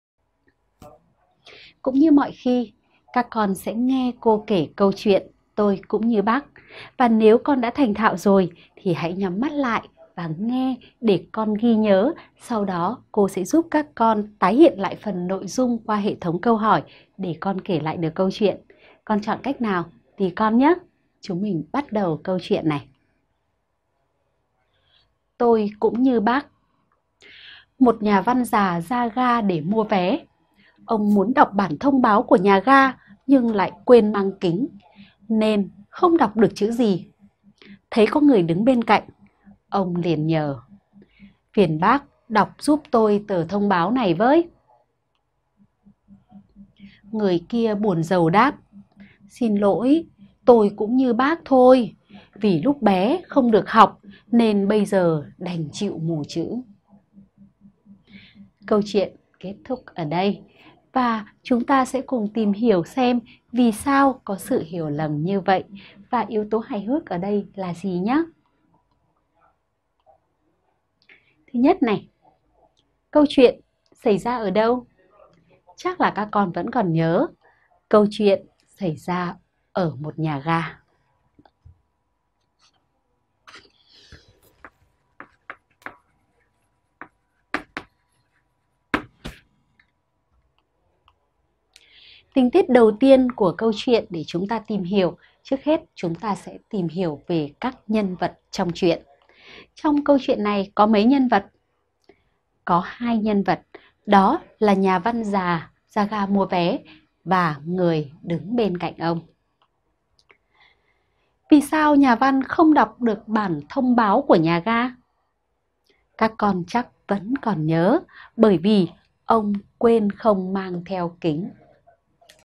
Tôi cũng như bác. Giới thiệu hoạt động (tiết 1) - Nghe – kể - Tập làm văn - Tuần 14 - Tiếng Việt 3
Đọc câu chuyện "Tôi cũng như bác" và trả lời câu hỏi:
         Một nhà văn già ra ga mua vé. Ông muốn đọc bản thông báo của nhà ga nhưng quên mang kính nên không đọc được chữ gì. Thấy có người đứng cạnh, ông liền nhờ:
Phiền bác đọc  giúp tôi tờ thông báo này với!
 Người kia buồn rầu đáp:
- Xin lỗi. Tôi cũng như bác thôi, vì lúc bé không được học nên bây giờ đành chịu mù chữ.
Câu hỏi:
+Câu chuyện xảy ra ở đâu ?
+Trong câu chuyện có mấy nhân vật ?
+Vì sao nhà văn không đọc được bản thông báo của nhà ga?
+Ông nói gì với người bên cạnh ?
+Người đó trả lời ra sao ?
+Người đứng bên đã hiểu nhầm điều gì ?
Tập làm văn: Nghe – kể: Tôi cũng như bác. Giới thiệu hoạt động (tiết 1)